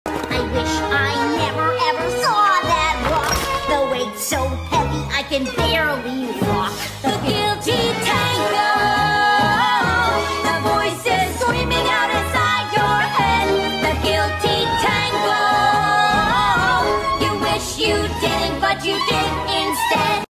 The Guilty Tango // FNAF: [ HUMAN AU ] // GC // Security Breach // TW: Blood (?)
ˋˏ ༻ ❛ ･ ❪ ❁ ❫ ･━━ ❜ ༺ ˎˊ

APPS USED:

- Gacha Club
- Kinemaster
- IbisPaint X
- CapCut

ˋˏ ༻ ❛ ･ ❪ ❁ ❫ ･━━ ❜ ༺ ˎˊ

- FNAF ALTERNATIVE UNIVERSE: [ HUMAN AU ]

All the animatronics are human in this world, but be warned, for not everything is as simple as it seems. Behind it’s fluffy exterior is a horror waiting to be uncovered.
Hope u enjoy the ride. :D

ˋˏ ༻ ❛ ･ ❪ ❁ ❫ ･━━ ❜ ༺ ˎˊ

VIDEO INFORMATION:

Chica has seen things that others have not.
She knows what Gregory is and is willing to do anything to protect her friends. (Not knowing that she's hurting them through her actions.)

 She believes she is the hero and will stop at nothing to get rid of the monster.

ˋˏ ༻ ❛ ･ ❪ ❁ ❫ ･━━ ❜ ༺ ˎˊ

MESSAGE:

Hello! I'm back with a vid. Hope you guys enjoy!
I think I'll be going for a once a week upload schedule since school is stacking up our workload.
The video basically summarised most of the events in the AU from Chica's perspective, but leaves a lot of the minor details out.

I'll see you guys in the next upload! (•u•)